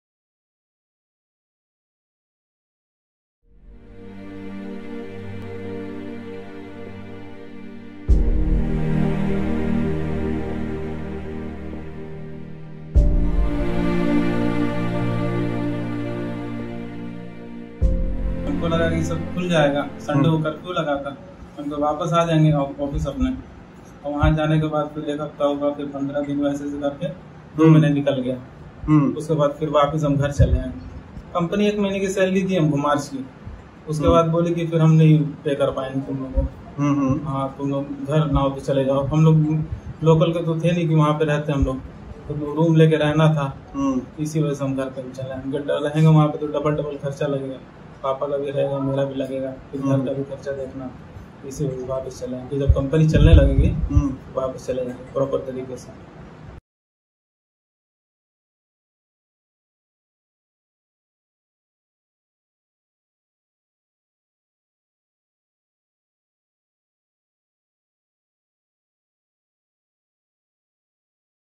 E01 : The Migrant Heroes: In Conversation with Afzal Mirza
The National Commission for Enterprises in the Unorganised Sector (NCEUS) reports around 92 percent of India's workforce with informal employment are substantially drawn from migrant labour.

This film is an outcome of an Ethnographic Study on migrants workers in Delhi, Haryana, Rajasthan, Uttar Pradesh, Bihar and Jharkhand, to observe excruciating condition migrants faced because of forced reverse migration caused by nationwide lockdown due to Covid-19 pandemic.